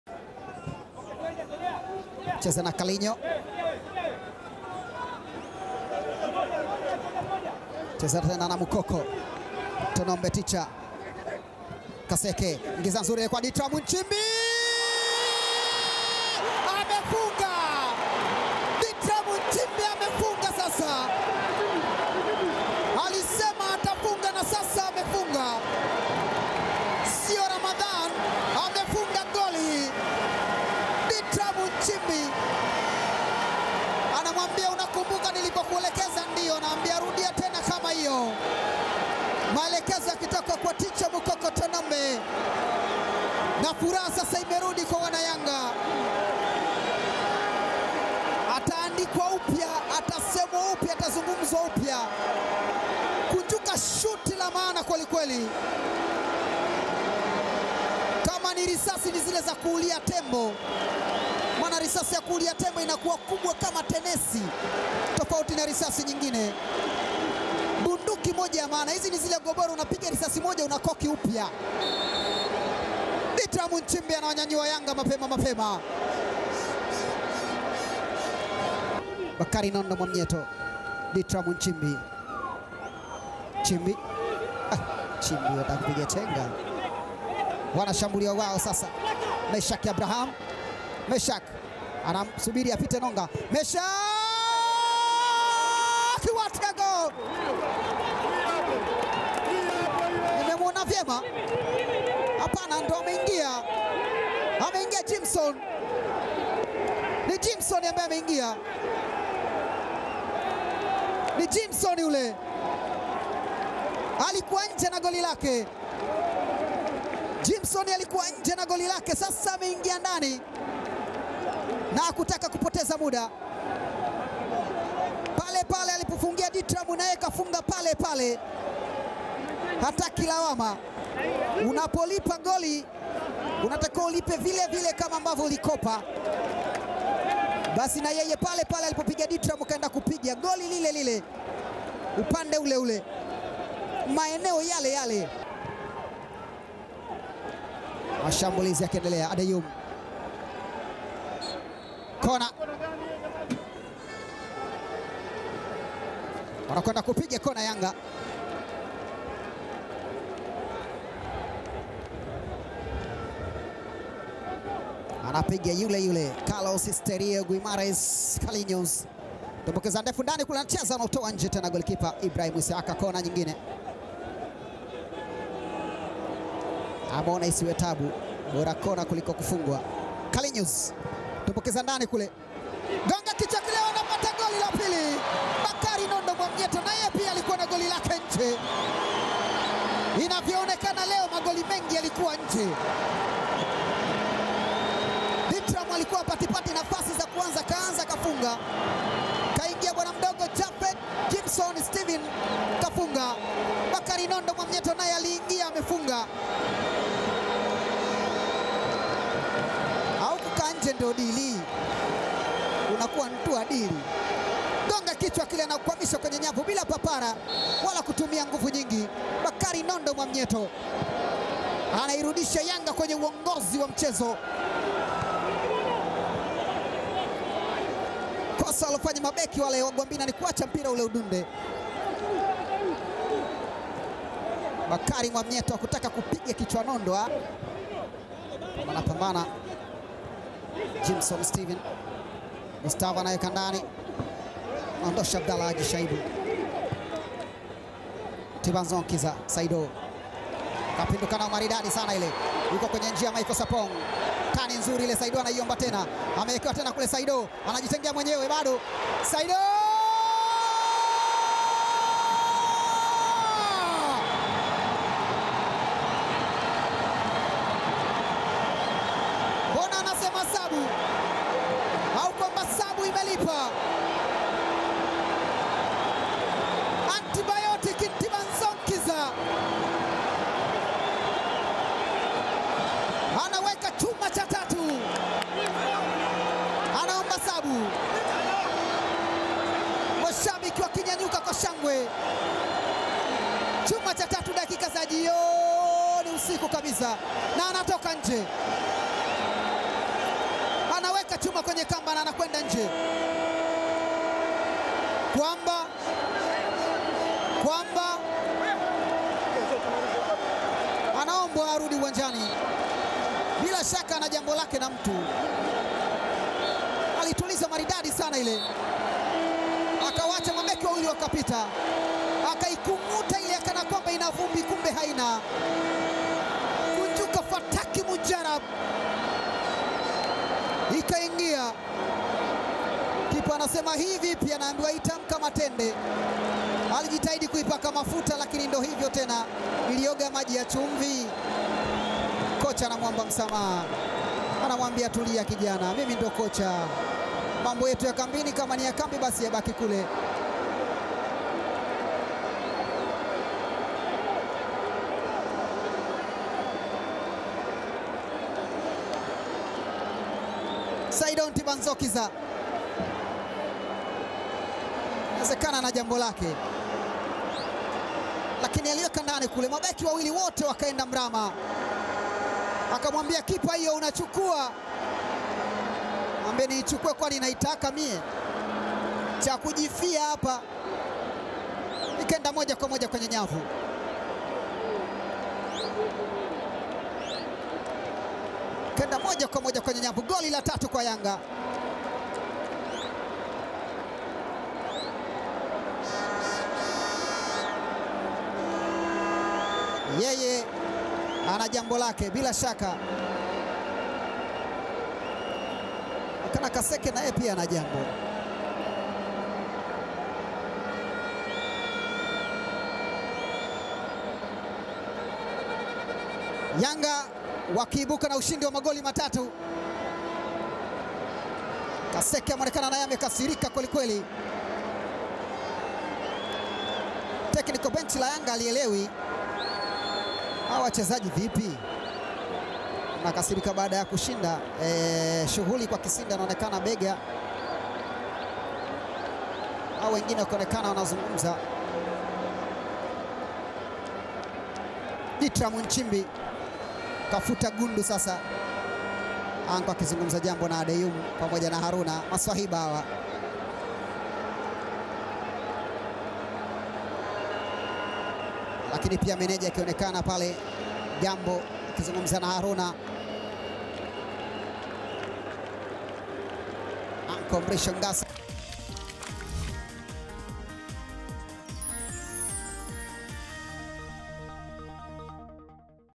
Yanga SC 3-1 Gwambina FC | Magoli yote na alichokifanya Ntibazonkiza | VPL 20/04/2021
Vijana wa Jangwani, Yanga SC wamepata ushindi mnono wa mabao 3-1 katika mchezo wa ligi kuu Tanzania Bara dhidi ya Gwambina FC uliochezwa leo kwenye Uwanja wa Benjamin Mkapa, Dar es Salaam.

Magoli ya Yanga yamefungwa na Ditram Nchimbi ambaye amefunga kwa mara ya kwanza tangu Februari 2020, Bakari Mwamnyeto na Saidi Ntibazonkiza huku goli pekee la Gwambina likifungwa na Jimson Stephen Mwanuke.

Tazama magoli hapa na kilichotokea baada ya Saidi kufunga goli la tatu ambalo pia ni la tatu kwake kwenye VPL msimu huu.